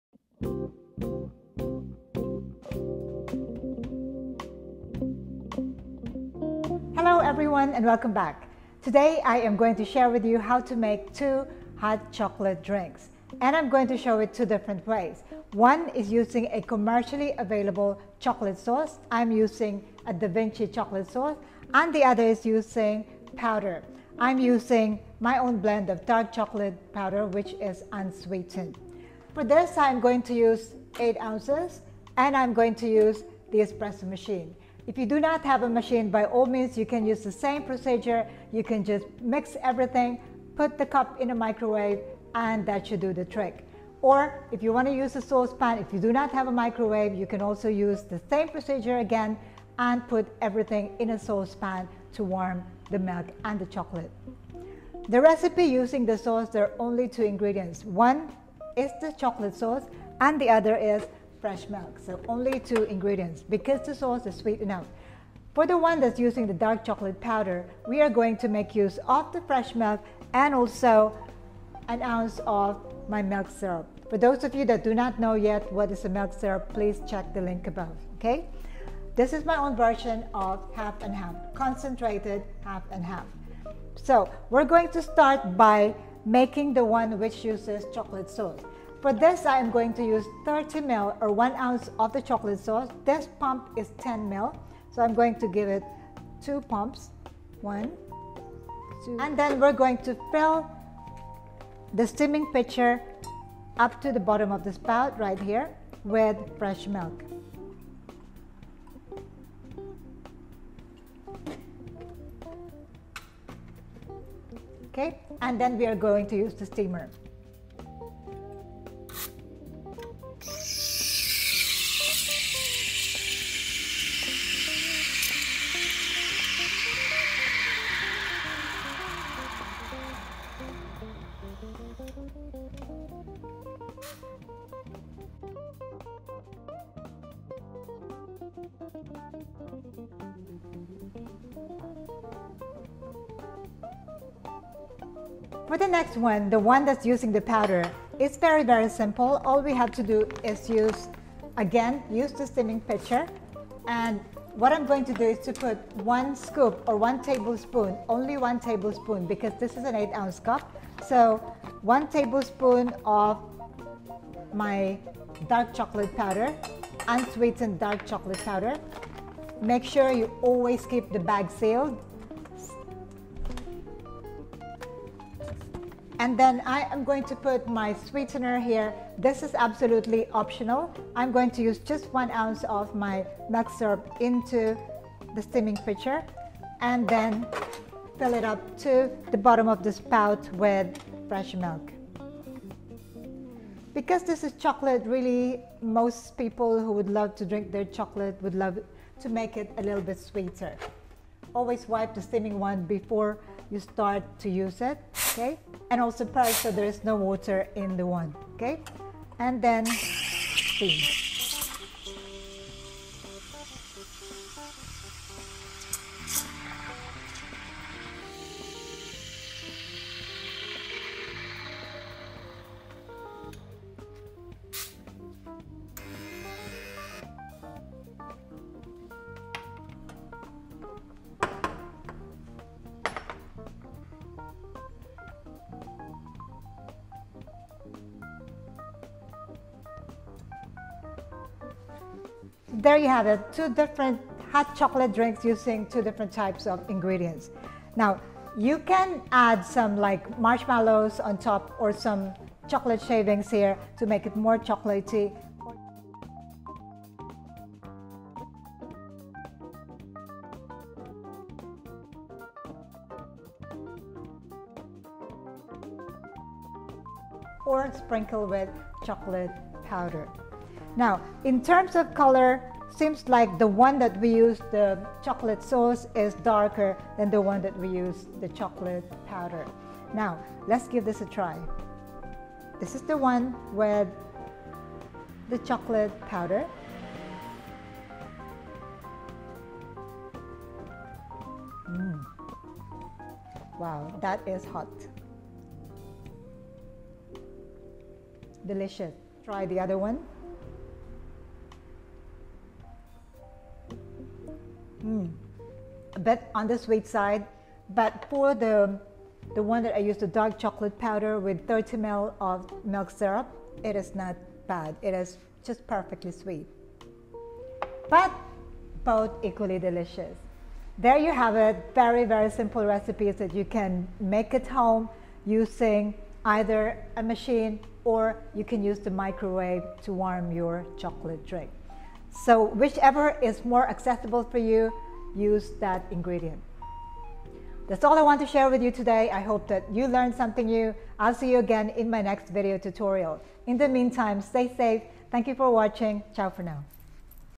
EASY HOT CHOCOLATE: 2 WAYS - SYRUP VS POWDER - WHAT'S THE DIFFERENCE?
1oz = 30ml

RECIPE FOR 8 OZ

Using Unsweetened Chocolate Powder
1 tbsp Unsweetened Dark Chocolate Powder
120-150ml Fresh Milk
30ml Milk Syrup

Using Commercial Chocolate Sauce
30ml Chococolate Sauce
120-150ml Fresh Milk